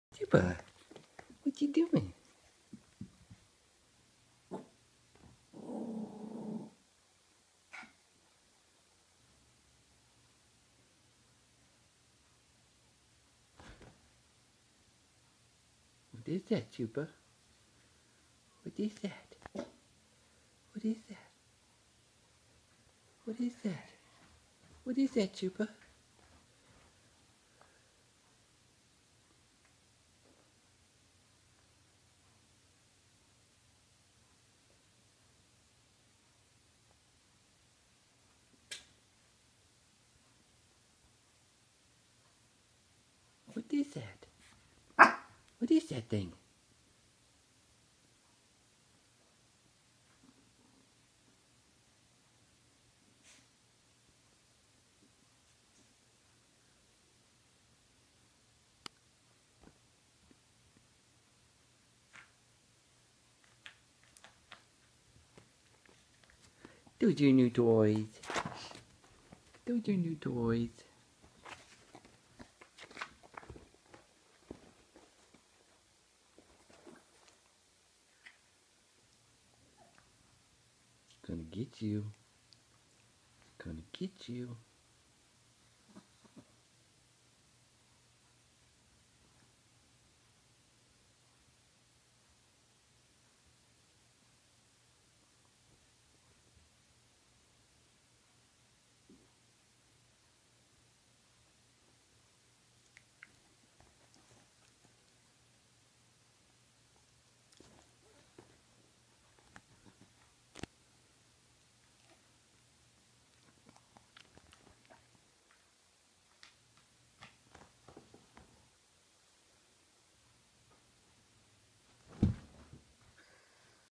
Chupa and the new toy skunk
Skunk only lasted an hour after this video was taken.